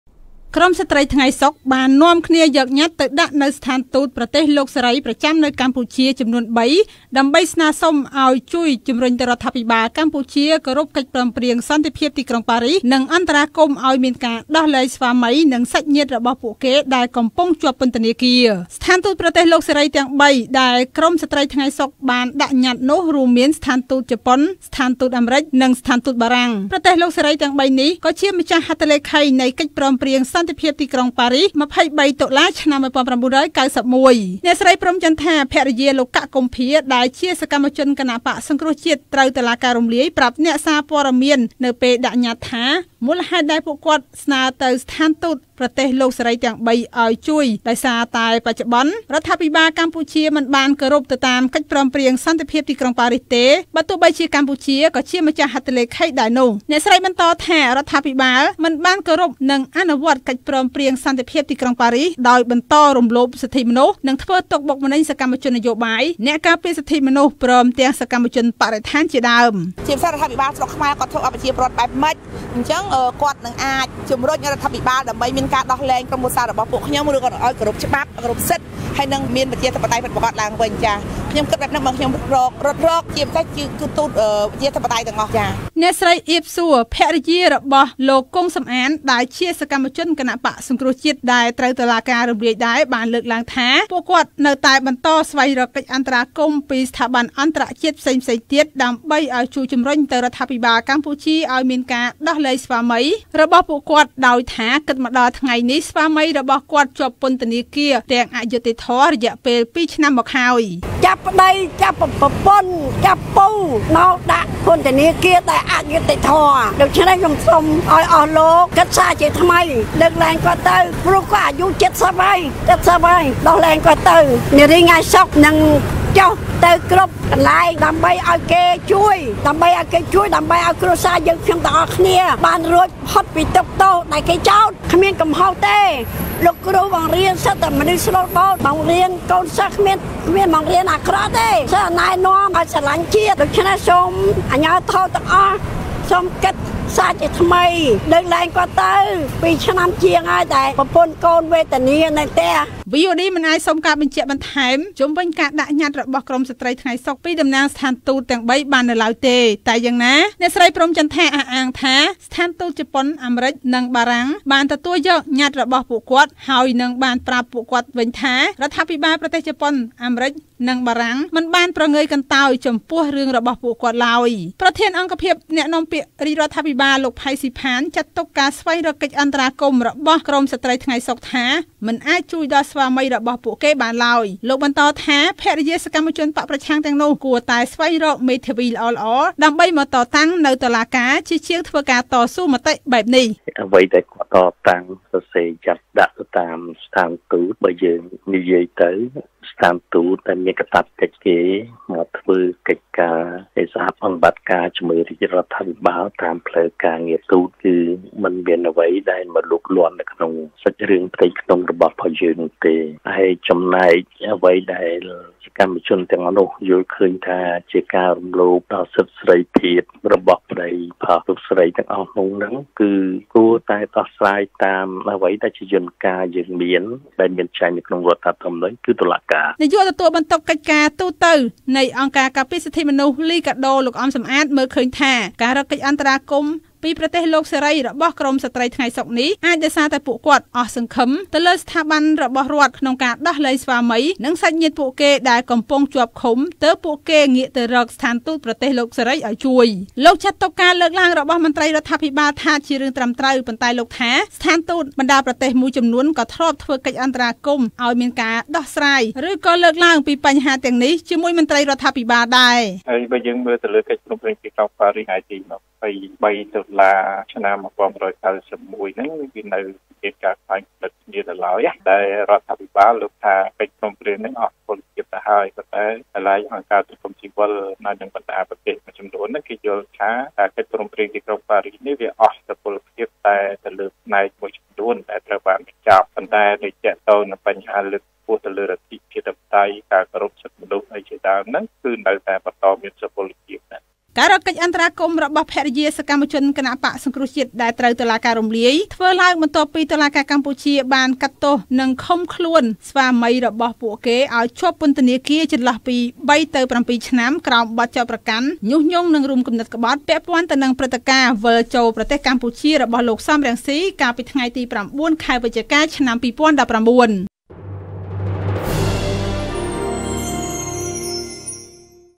ស្រ្តីថ្ងៃសុក្រស្នើស្ថានទូត៣ ជួយជំរុញរដ្ឋាភិបាលកម្ពុជាដោះលែងគ្រួសារ
ស្រ្តីថ្ងៃសុក្រស្នើស្ថានទូត៣ ជួយជំរុញរដ្ឋាភិបាលកម្ពុជាដោះលែងគ្រួសារ និងគោរពកិច្ចព្រមព្រៀងសន្តិភាពទីក្រុងប៉ារីស
រាយការណ៍បន្ថែមដោយកញ្ញា ខាន់ លក្ខិណា